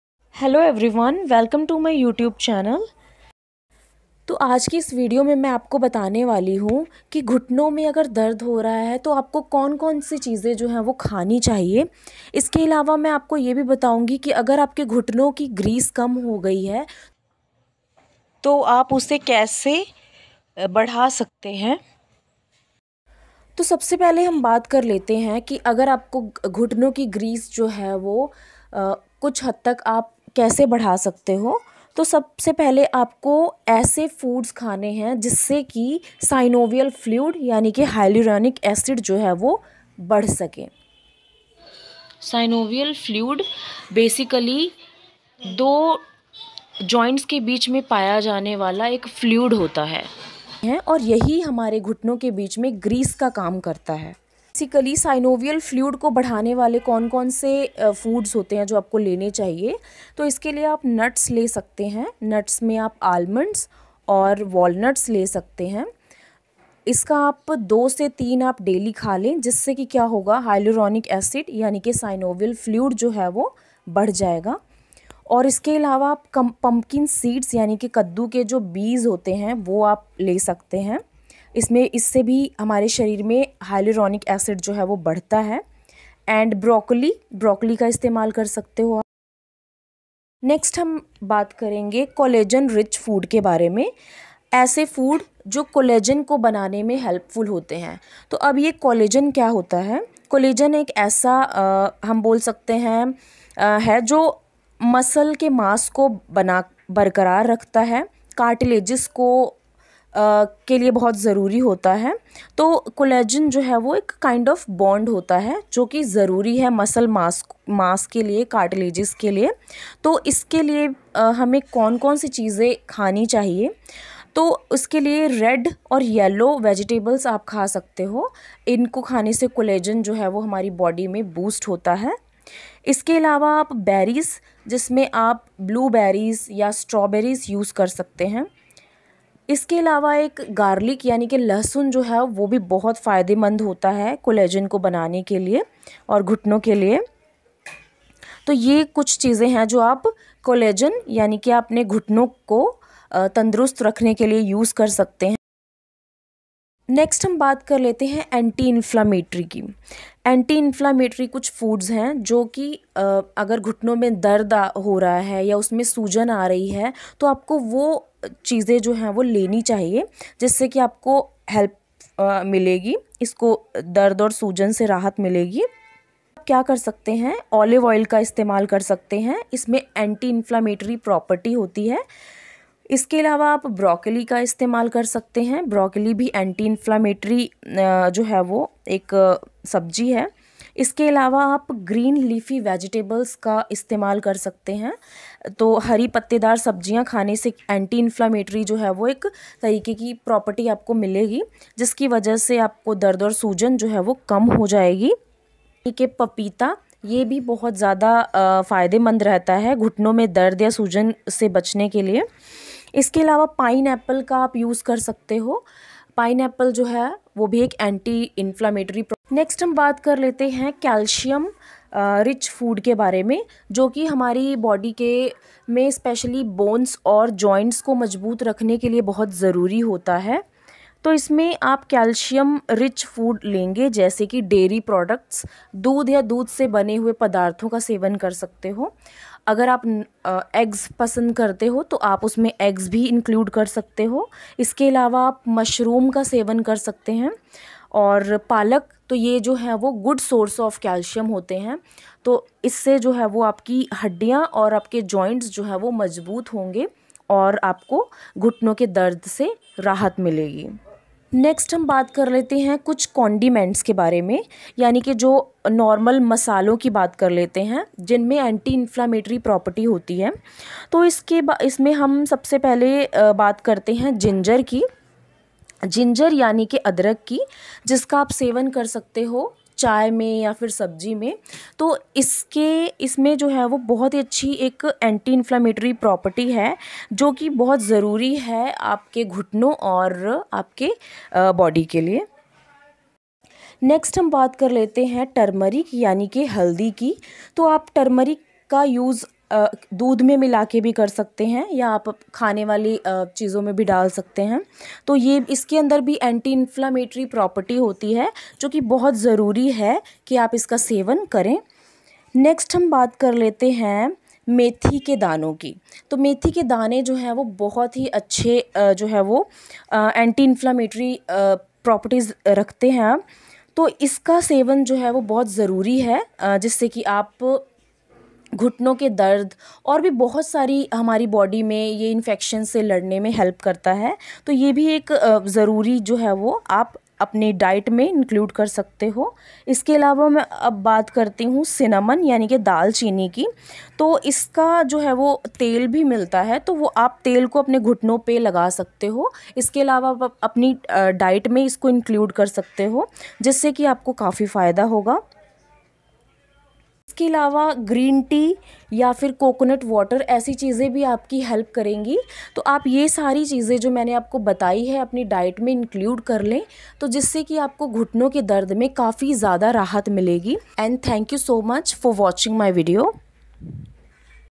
घुटनों के दर्द में और घुटनों की ग्रीस बढाने के लिए क्या खाएं
Hello everyone welcome to my YouTube channel Aaj main aap sabko btane vali hu ghutno ki greece ko badhane k liye kya khaye aur ghutno me dard hai to kya khana chahiye. I hope ye video apke liye useful ho